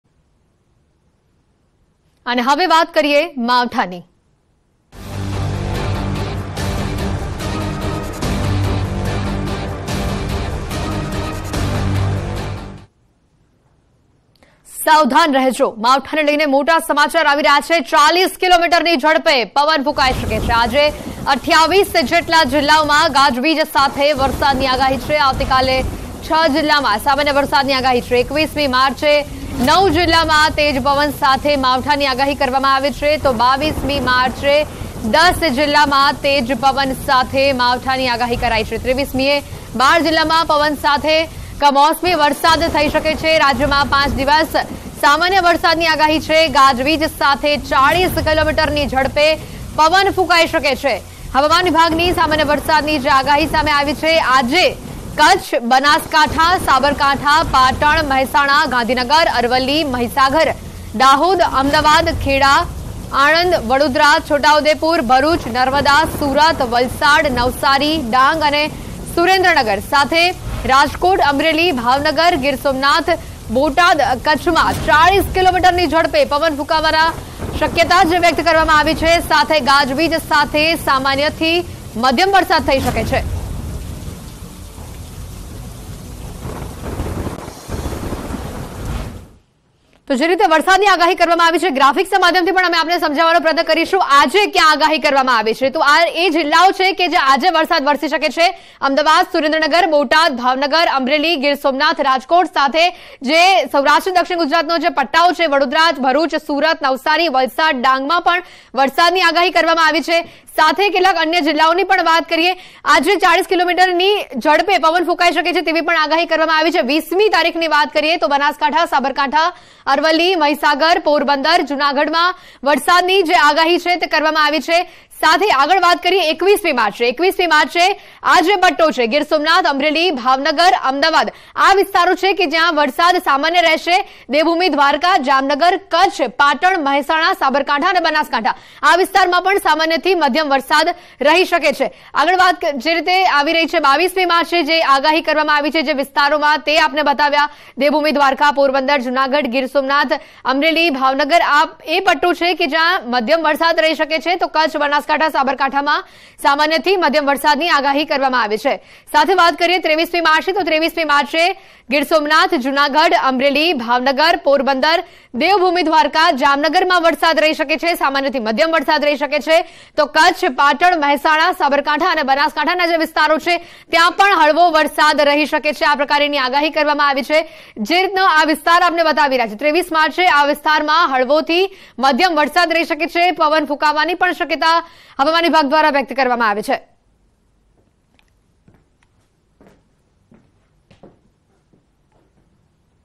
Unseasonal Rain : માવઠાને લઇ મોટા સમાચાર | Gujarat Weather Update | News In Gujarati
Big news about Mawtha.  Wind speed of 40 kmph may blow in the state.

માવઠાને લઇ મોટા સમાચાર. રાજ્યમાં ફૂંકાઈ શકે છે 40 કિમીની ઝડપે પવન.n18oc_gujarat